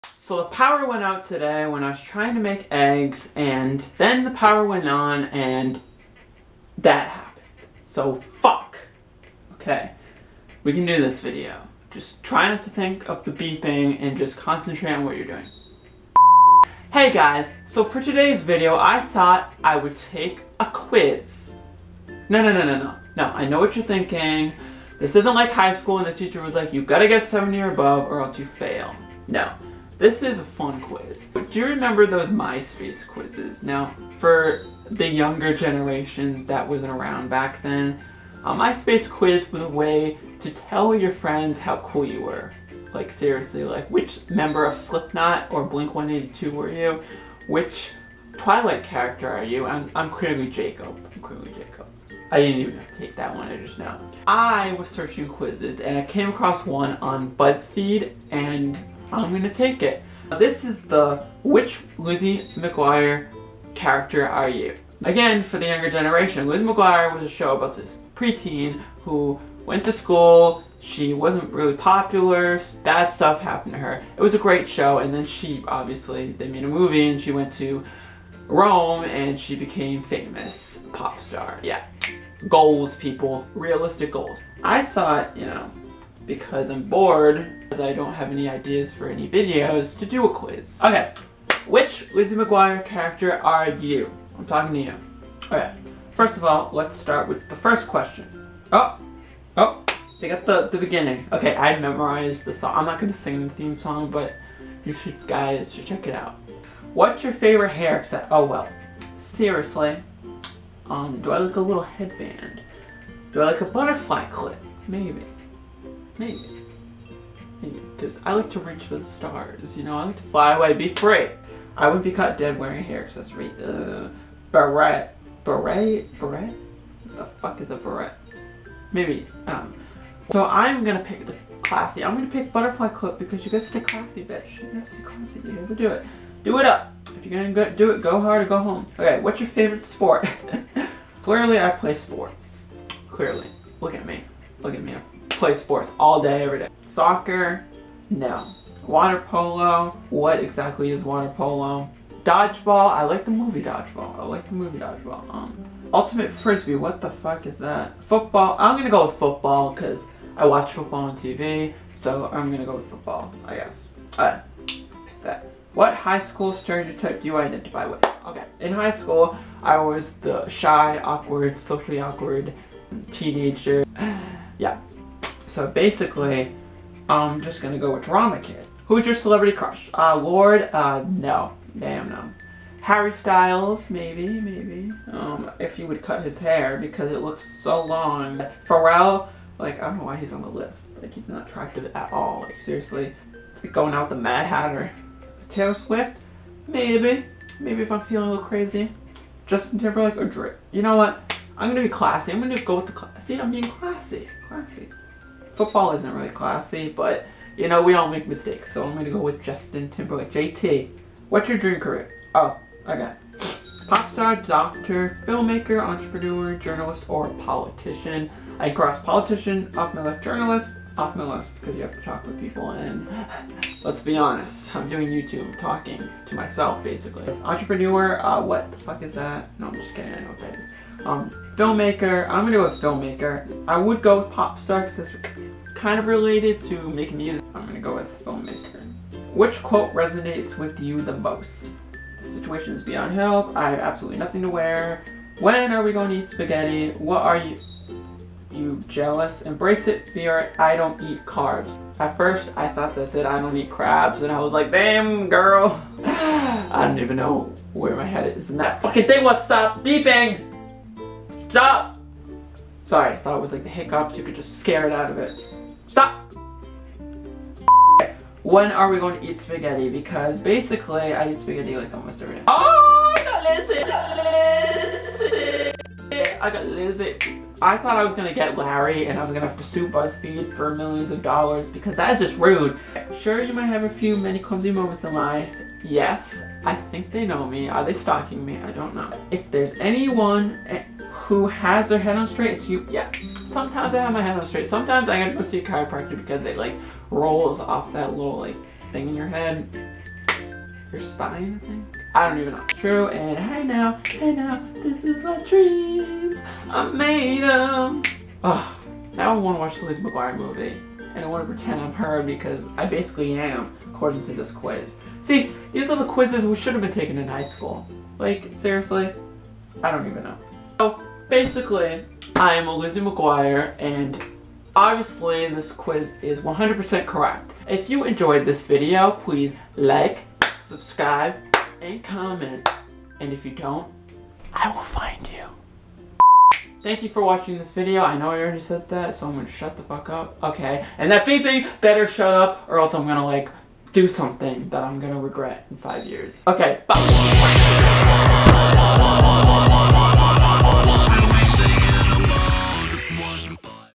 Lizzie McGuire Nudes Leak | Dylan Cataldo
Hello youtubers... Can you believe that Hilary Duff's nudes have leaked. I am sure it does not surprise anyone. After all she is a Disney Star. Just look at Miley Cyrus. It's crazy what is happening to a lot of celebs nowadays. Anyways, I am going to take this quiz to find out which Lizzie McGuire character I am. This should be fun... Although I did hate taking quizzes in High School.